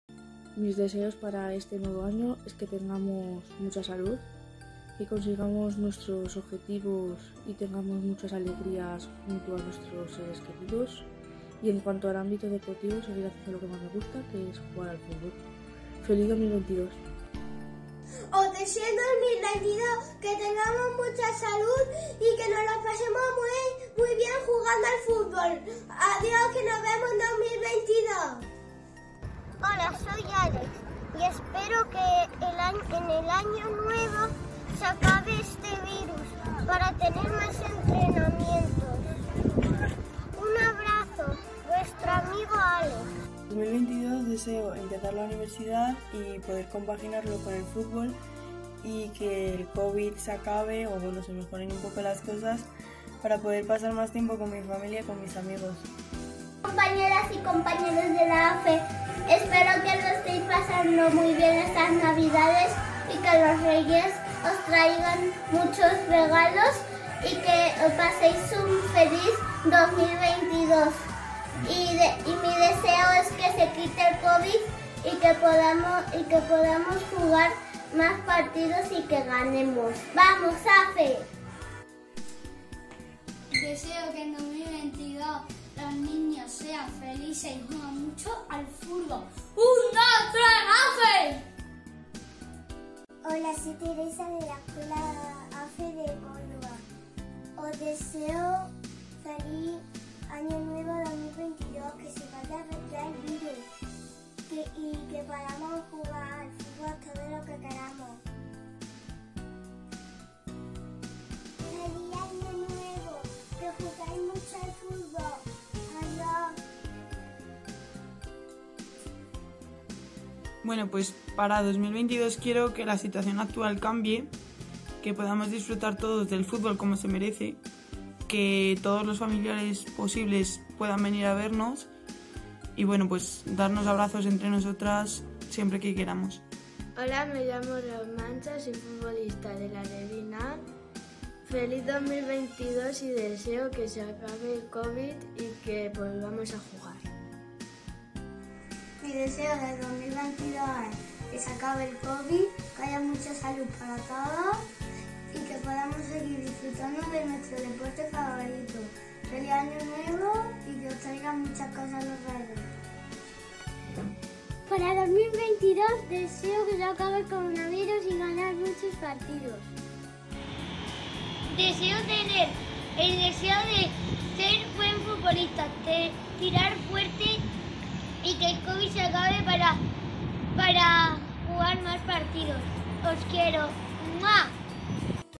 Los deseos de los alumnos de las Escuelas AFE para el 2022
Todos coinciden en una cosa... ¡qué se termine el covid!